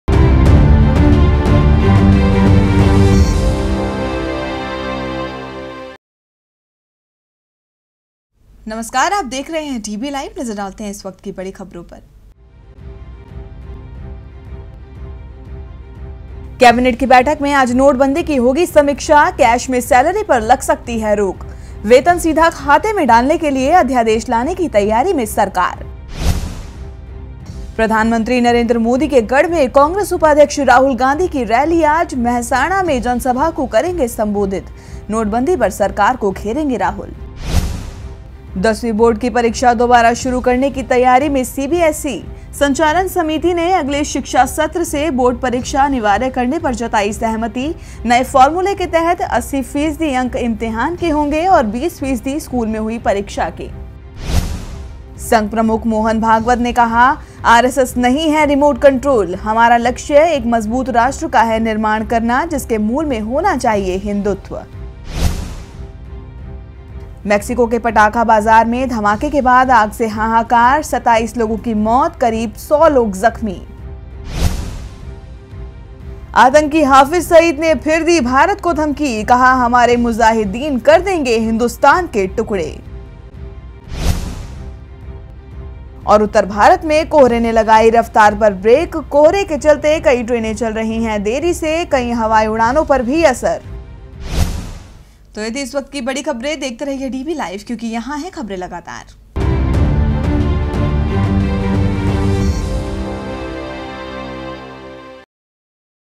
DB LIVE | 21 DEC 2016 | News Headlines
1..कैबिनेट की बैठक में आज नोटबंदी की होगी समीक्षा,,,कैश में सैलेरी पर लग सकती है रोक...वेतन सीधा खाते में डालने के लिए अध्यादेश लाने की तैयारी में सरकार।
कैश में सैलेरी पर लग सकती है रोक
2..प्रधानमंत्री नरेंद्र मोदी के गढ़ में कांग्रेस उपाध्यक्ष राहुल गांधी की रैली आज. मेहसाणा में जनसभा को करेंगे संबोधित..नोटबंदी पर सरकार को घेरेंगे राहुल।
गुजरात में गरजेंगे राहुल
3.. दसवीं बोर्ड की परीक्षा दोबारा शुरू करने की तैयारी में सीबीएसई... संचालन समिति ने अगले शिक्षा सत्र से बोर्ड परीक्षा अनिवार्य करने पर जताई सहमति...नए फॉर्मूले के तहत 80 फीसदी अंक इम्तिहान के होंगे और 20 फीसदी स्कूल में हुई परीक्षा के।
फिर शुरू होगी 10वीं बोर्ड की परीक्षा
4...संघ प्रमुख मोहन भागवत ने कहा, आरएसएस नहीं है रिमोट कंट्रोल...हमारा लक्ष्य एक मज़बूत राष्ट्र का है निर्माण करना...जिसके मूल में होना चाहिए हिन्दुत्व।
‘RSS नहीं है रिमोट कंट्रोल’
5..मेक्सिको के पटाखा बाज़ार में धमाके के बाद आग से हाहाकार...27 की मौत, करीब 100 लोग ज़ख्मी।
मेक्सिको में जानलेवा अग्निकांड
6..आतंकी हाफिज़ सईद ने फिर दी भारत को धमकी...कहा, हमारे मुज़ाहिद कर देंगे हिन्दुस्तान के टुकड़े।
आतंकी हाफिज़ सईद की गीदड़भभकी
7..उत्तर भारत में कोहरे ने लगाई रफ्तार पर ब्रेक...कोहरे के चलते कई ट्रेनें चल रही है देरी से...कई हवाई उड़ानों पर भी असर।
कोहरे ने रोकी रफ्तार